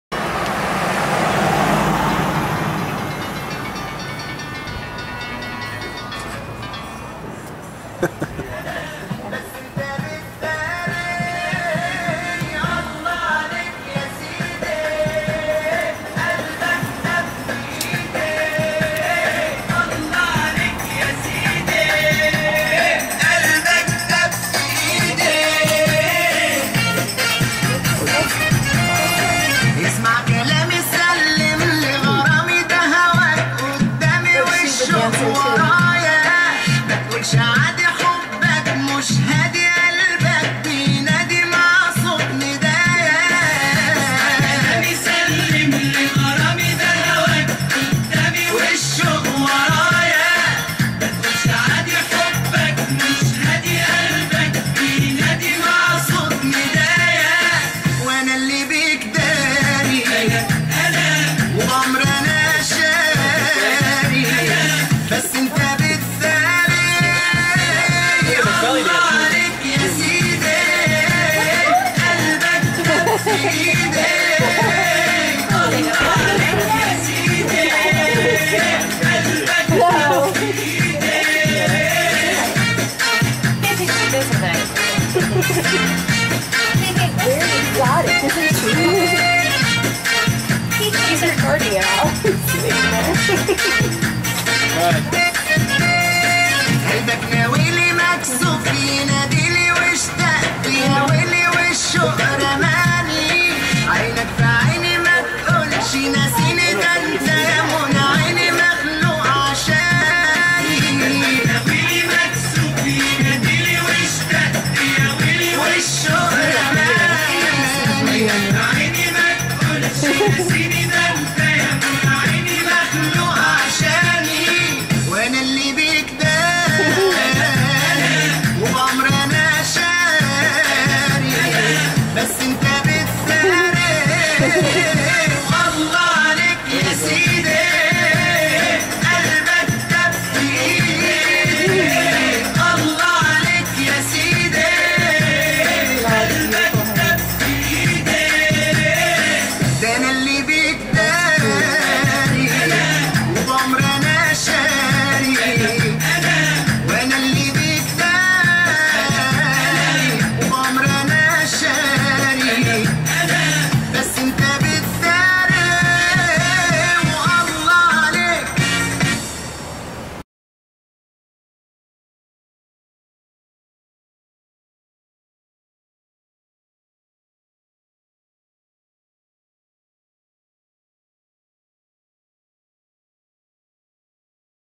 Traverse City Goes International
Walking passed Zakey's Restaurant on Front Street in Downtown Traverse City, we were entertained by the marvelous dancing of Kimmy O, as we stood outside on the sidewalk. Filmed by Wayne Erreca (12-30-11)